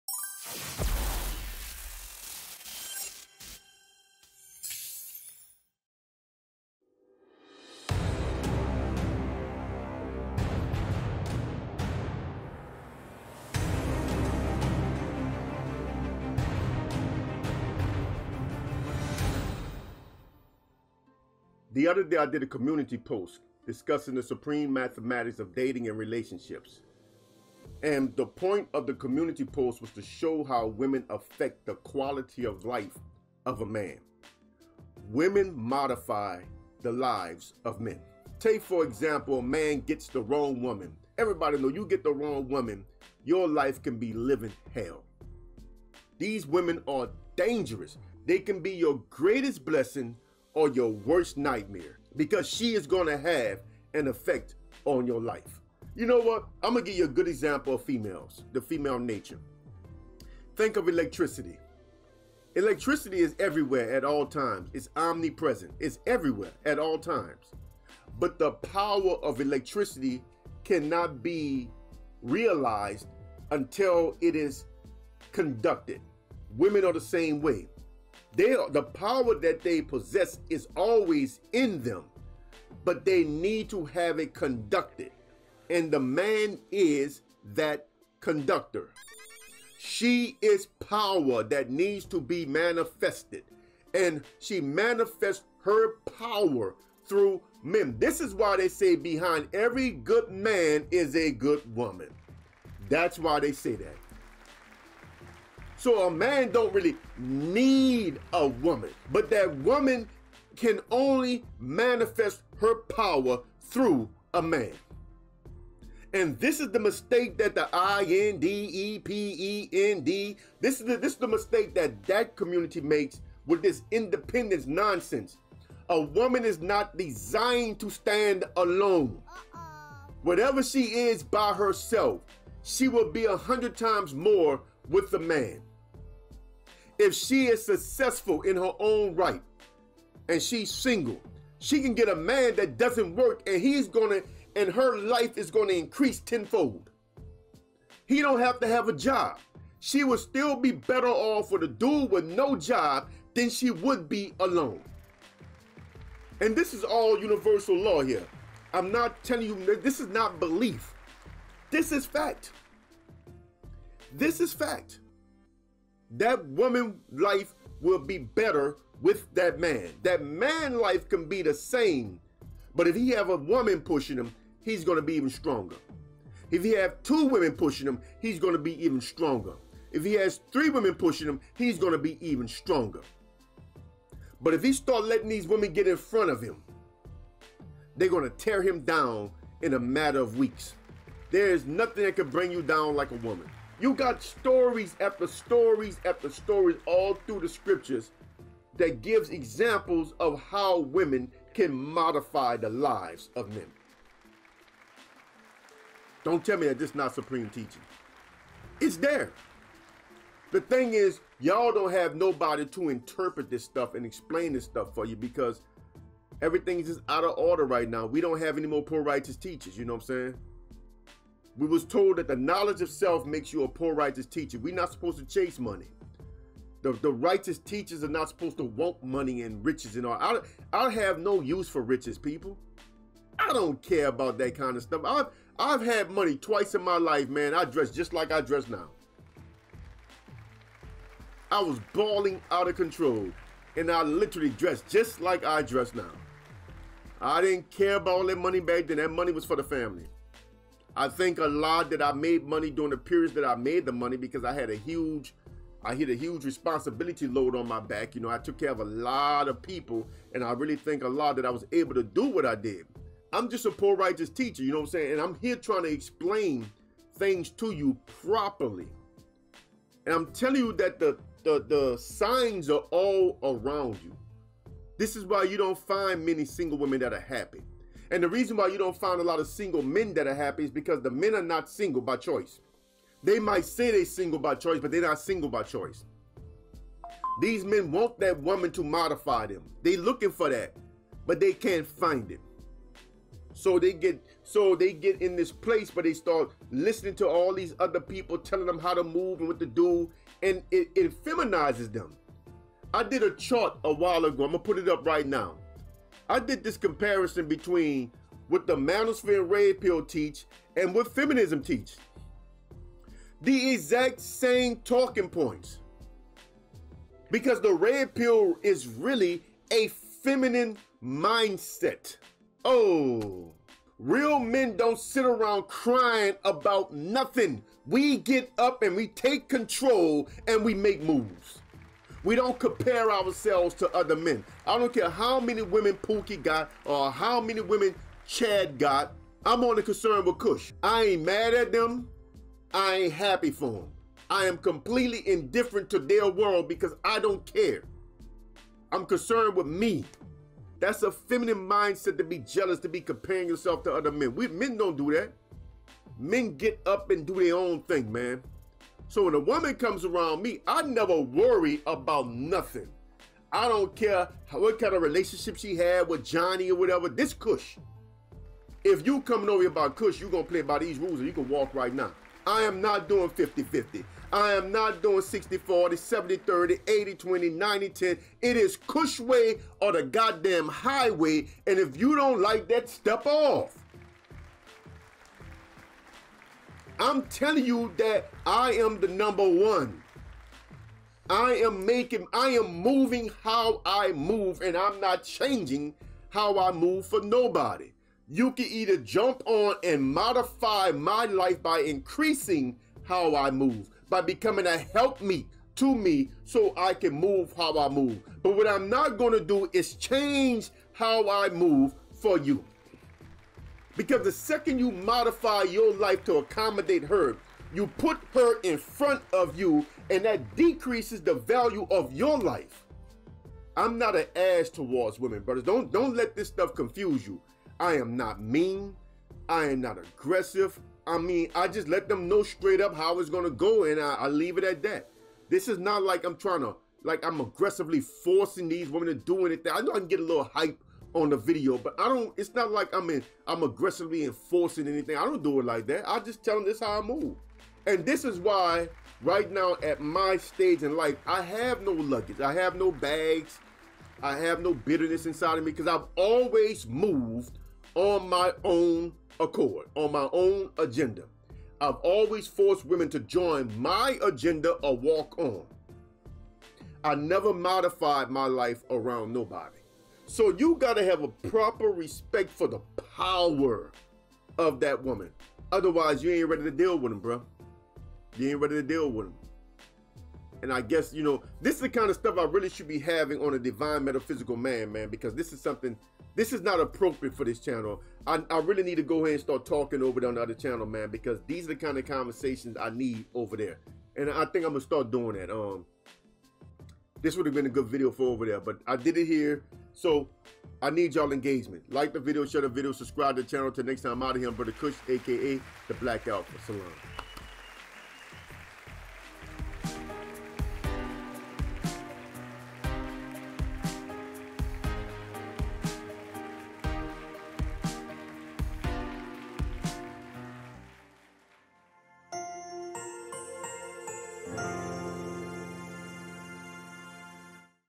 Bro. Kush on HOW WOMEN AFFECT THE LIVES OF MEN (Supreme Teachings)
MY COPYRIGHT PROTECTION:
CONTENT ADVISORY:
This video is suitable for some minors. Some strong language used, but NO sexually explicit images are used, in this video.